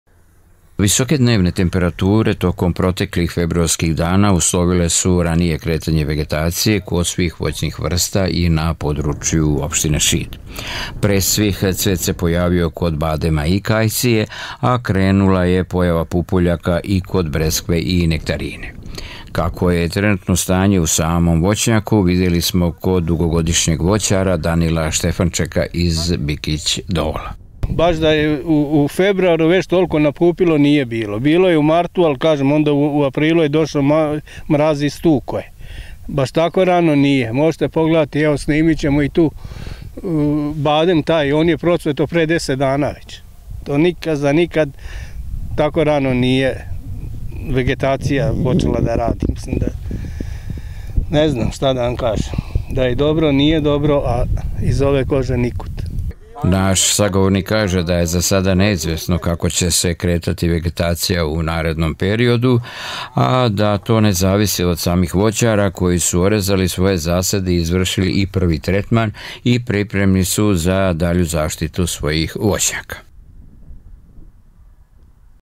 Prerano kretanje vegetacije
Pod višegodišnjim zasadima u opštini Šid nalazi se oko 400 hektara raznih voćnih vrsta, od kojih su najzastupljenije jabuke, breskve i nektarine. Protekli period toplog vremena uz visoke dnevnih temperatura uslovio je ubrzano kretanje vegetacije kod svih voćnih vrsta. Posebno je to primetno kod kajsije i badema.